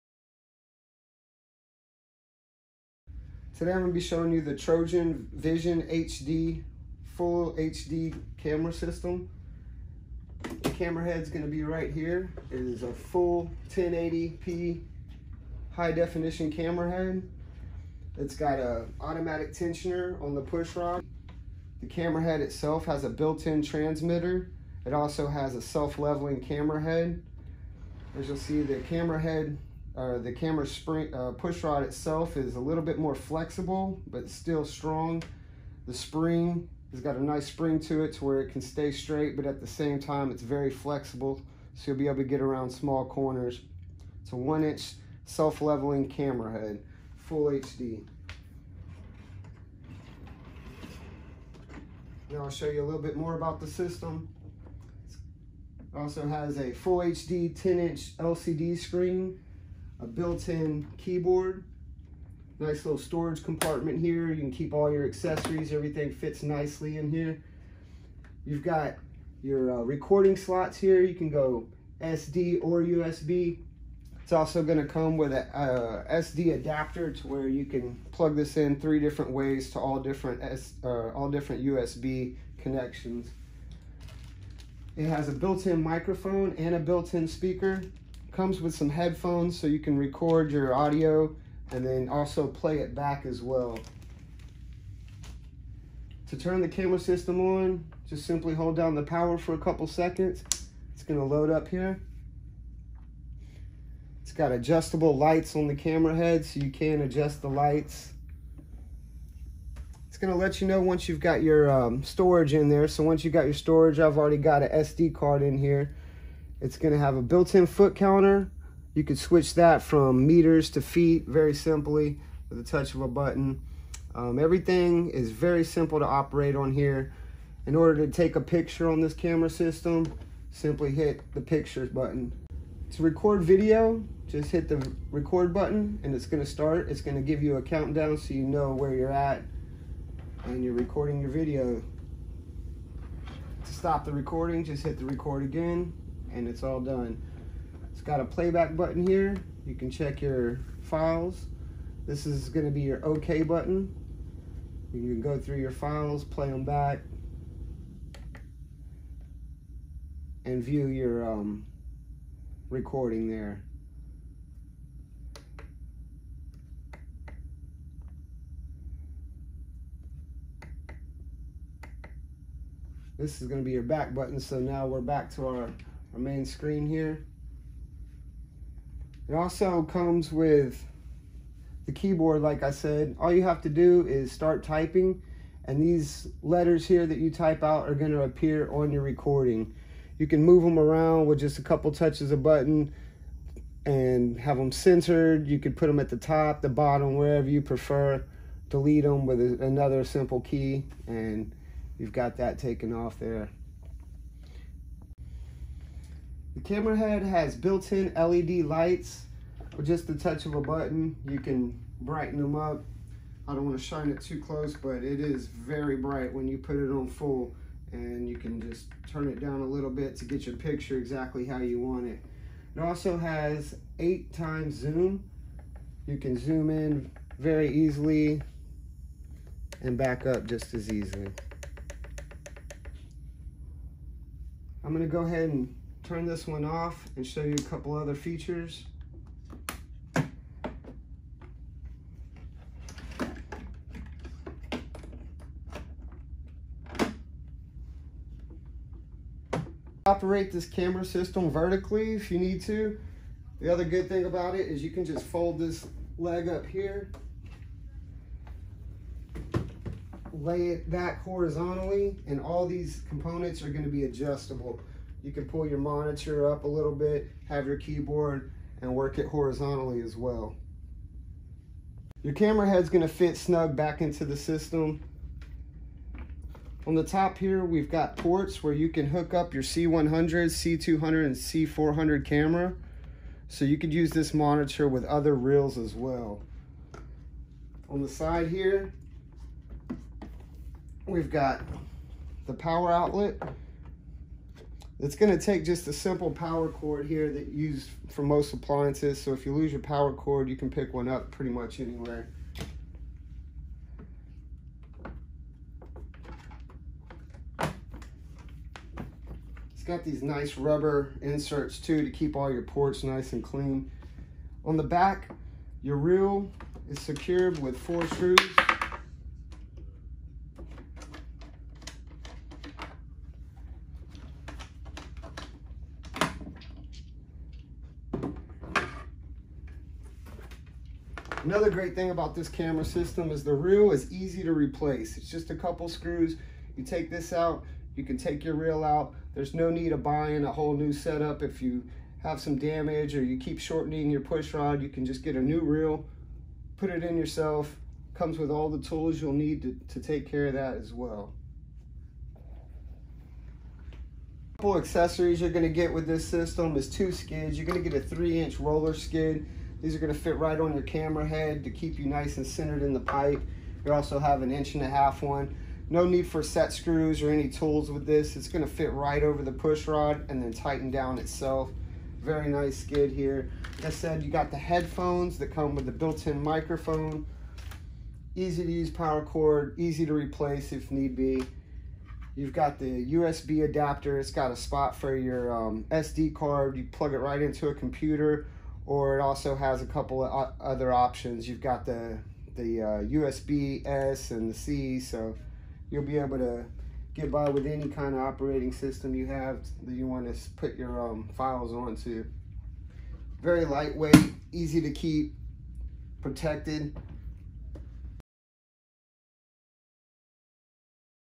Trojan VisionHD 1080P Camera System
-1-1/2" - 4" Lines
-1" 1080P High Definition Self Leveling Color Camera Head with 8X Zoom Feature
-Spring with Built in 512Hz Transmitter
-10.2" LCD Monitor with DVR, Speaker and Microphone for Voice Recording
-Keyboard for Text Writing
-100' of 3/8" Flexible Push Rod to Easily Maneuver Bends in Any Pipe
-Durable Protective Case with Partitioned Reel Housing for Quick Removal and Cleaning
-Three Additional Input Connections For Use with the 200', 400' and Pan & Tilt Reels
-Works with AC and DC Power (7hr Battery Life)
-Includes: 2 Skids with Quick Change Adaptor
64gb SD Card with USB, Micro USB and USB-C Adaptor and Charging Cord